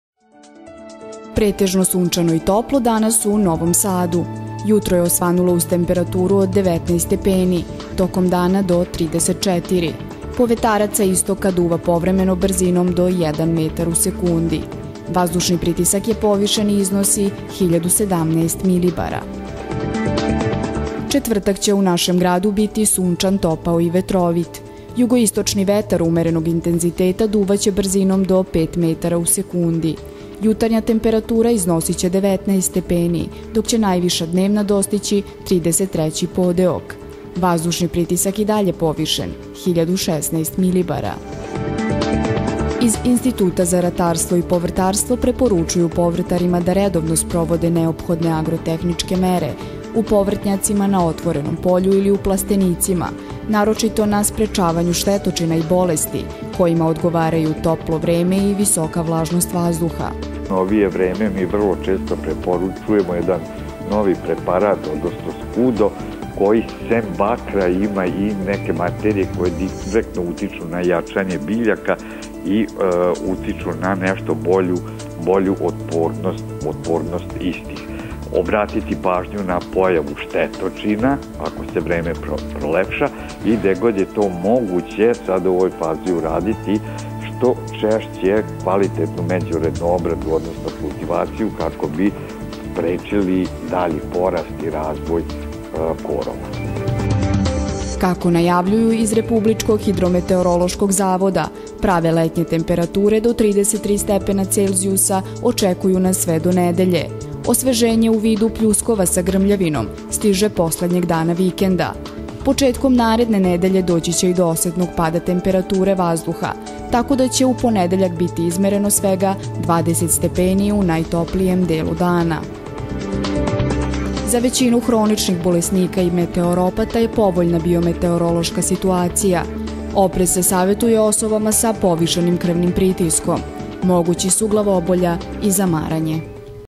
TV KANAL 9, NOVI SAD: VREMENSKA, AGRO I BIOPROGNOZA, 22.8.2018.
U Novom Sadu još jedan dan sa pravim letnjim temperaturama. Slično vreme očekuje nas do nedelje, za kada nam meteorolozi najavljuju osveženje.